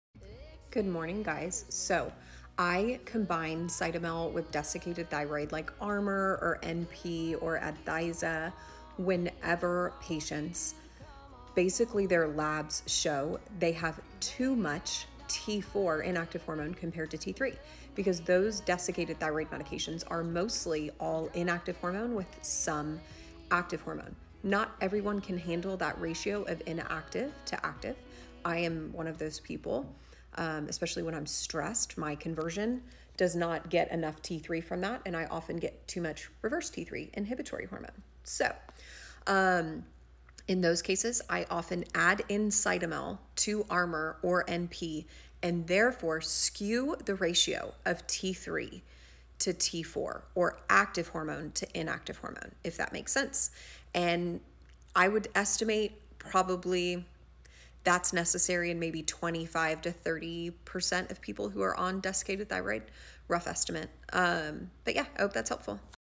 Combining Cytomel with Desiccated Thyroid
I get this question a lot: is it ever appropriate to add Cytomel or Liothyronine in with natural desiccated thyroid medication? Here’s the details on when I use that and the reasons behind why.